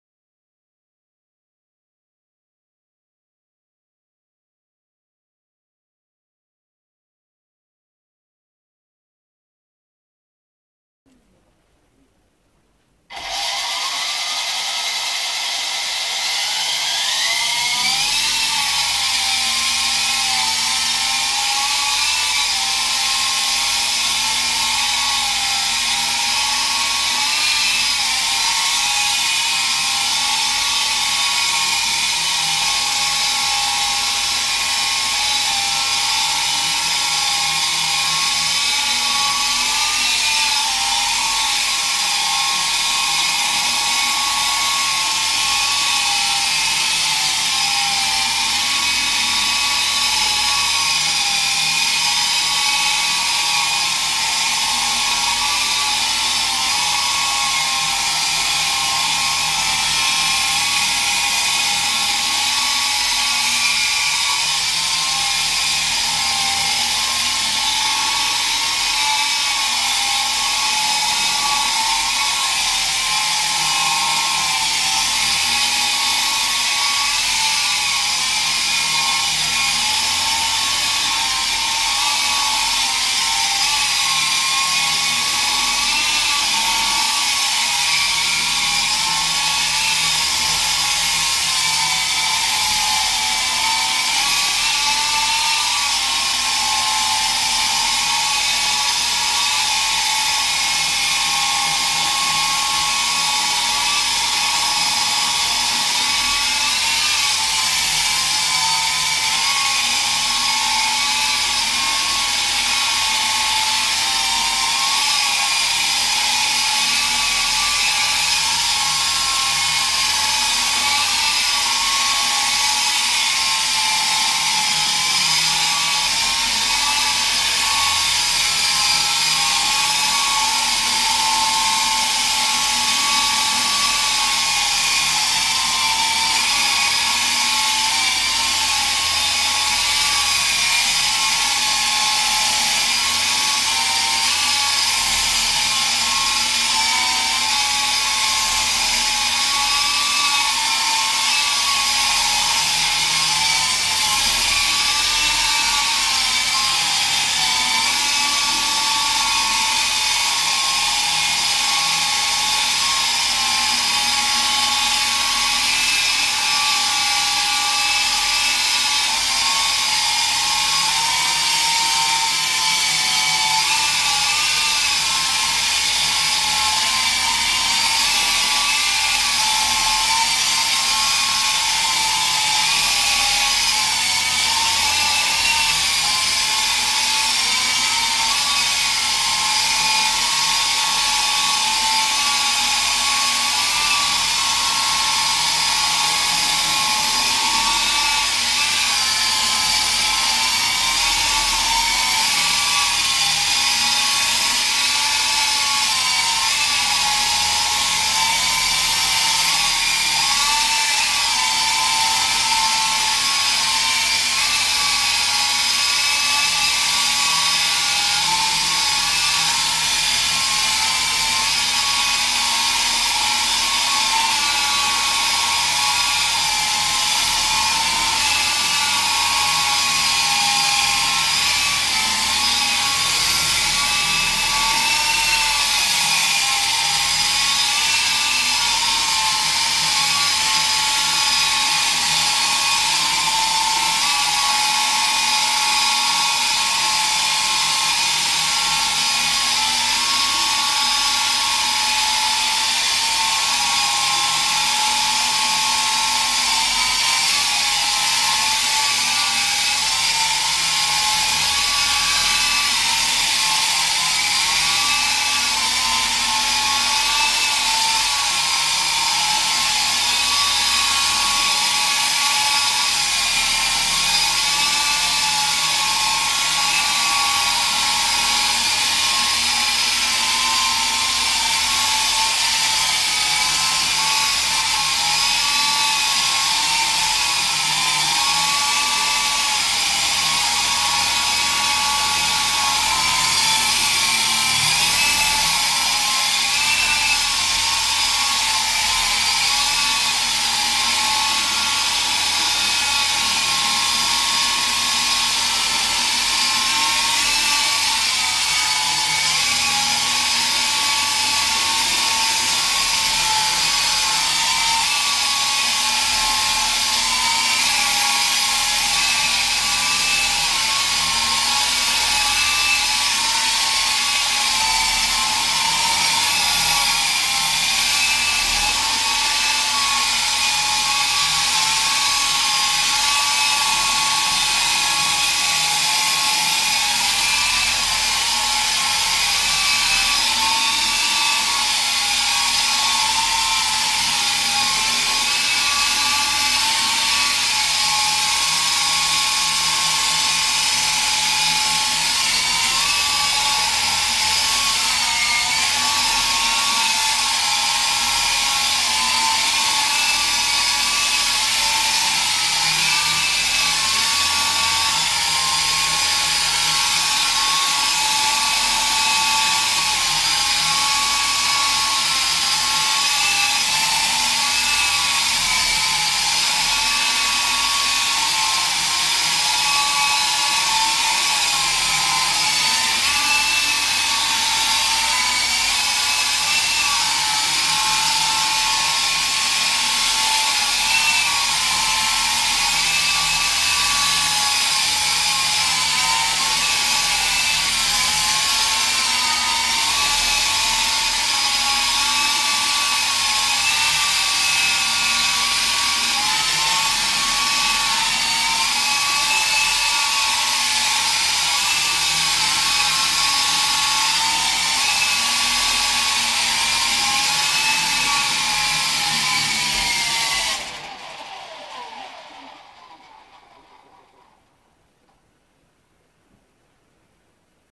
T-34 HELICOPTER INDOOR FLYING WITH BLACK CANOPY
This is the canopy that was damaged in my video with the spy cam. I bought a new one but decided to glue it back together and spayed it black, i think it looks pretty cool.